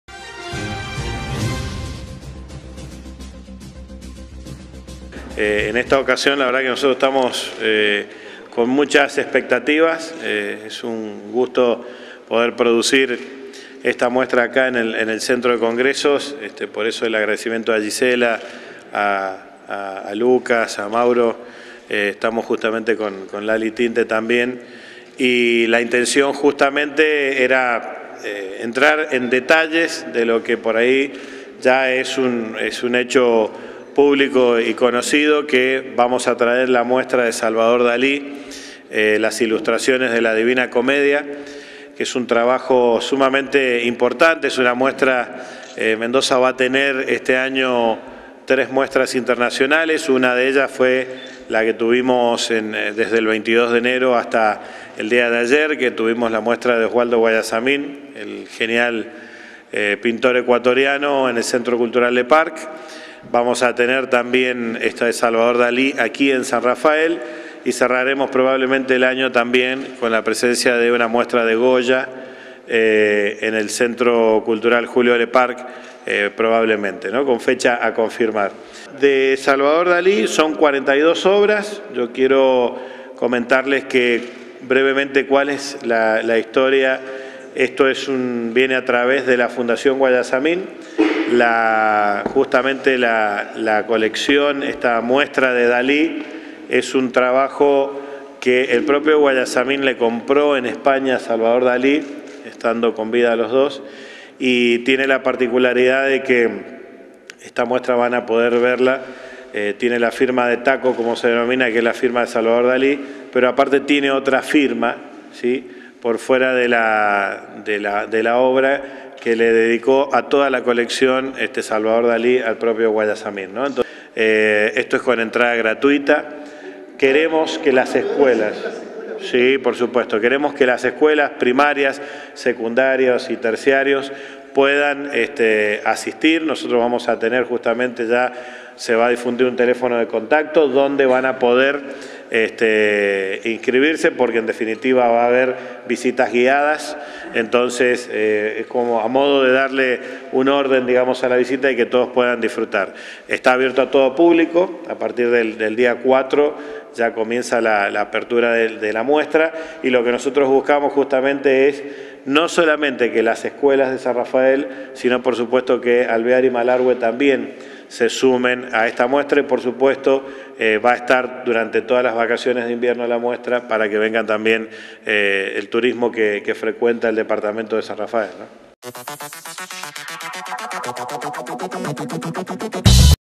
GENIAL MUESTRA PLÁSTICA
La Secretaría de Cultura de la Provincia de Mendoza s encuentra abocada a las presentaciones de una muestra plástica del genial Dalí, en una recorrida por todo el territorio provincial. En esta ocasión, los sanrafaelinos tendremos la oportunidad de gozarnos con las maravillosa e históricas obras de Dalí, durante unos diez días que estará la muestra en los salones del Cetro de Exposiciones y Congresos 'Alfredo Bufano' de San Rafael.